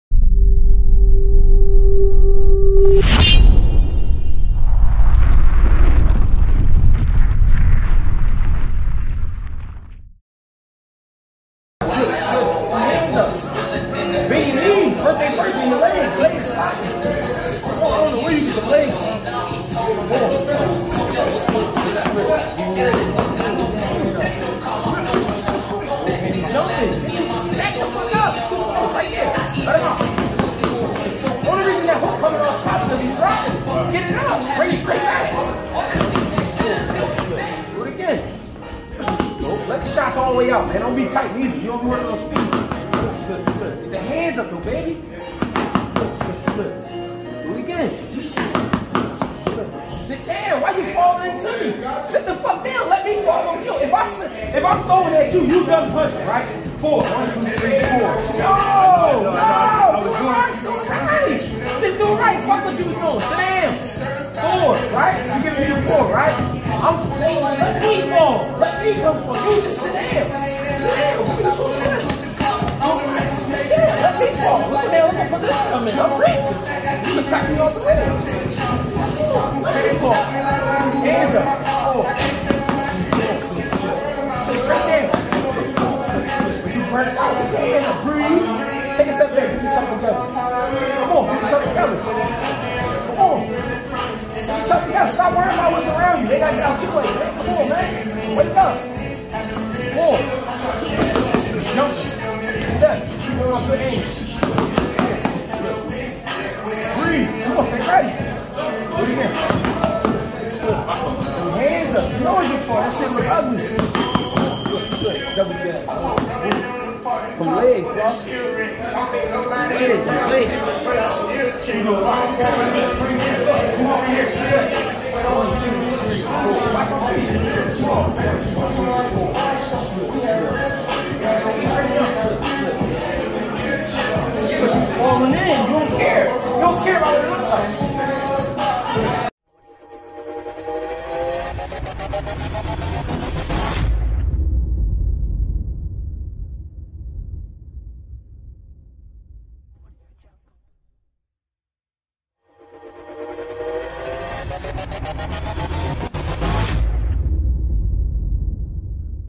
Greg Hackett gets INTENSE during training!!!!
Trainer Greg Hackett holds nothing back when training his fighter at Philly 1on1 boxing gym in Philadelphia PA .